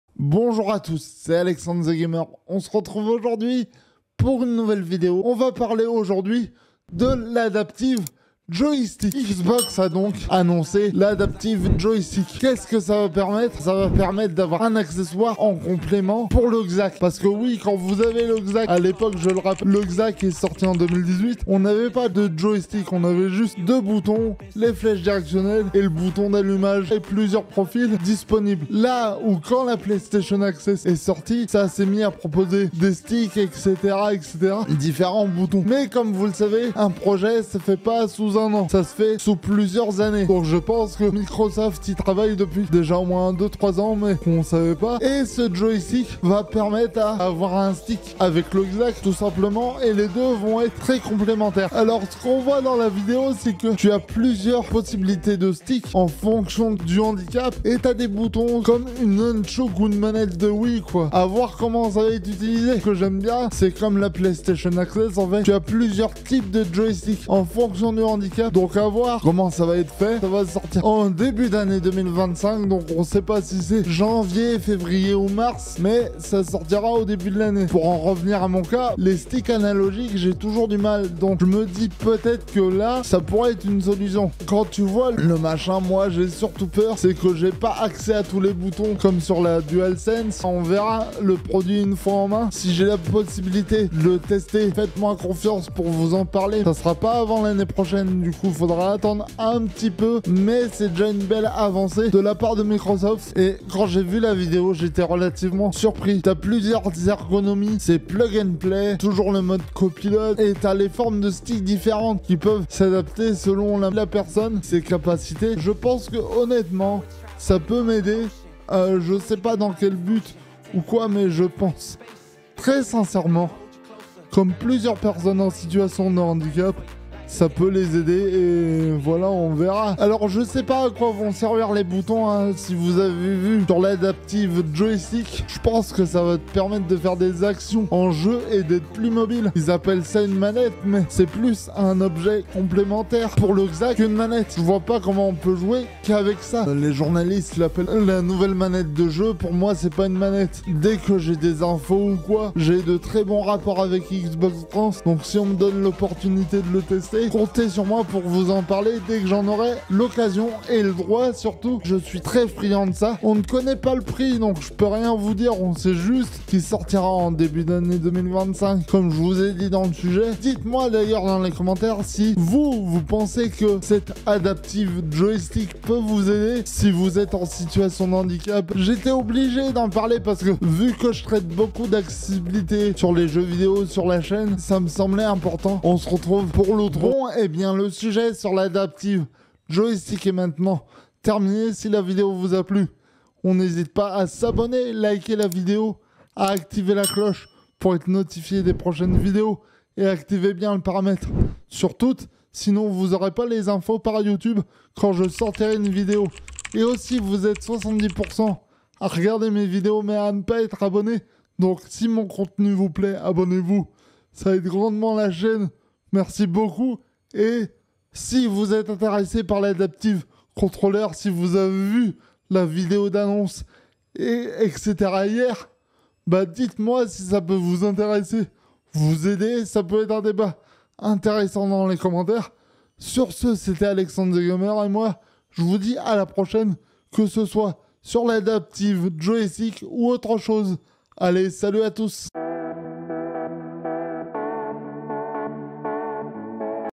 XBOX ANNONCE L'ADAPTIVE JOYSTICK !
Xbox en amont de la Gamescom a annoncé l'adaptive joystick un accessoire pour  les personnes en situarion de handicap en plus du Xbox Adaptive controller sorti  en 2018 une réelle avancée

L'adaptive joystick sortira début 2025

Qu'est ce que ça peut apporter ? on voit ça ensemble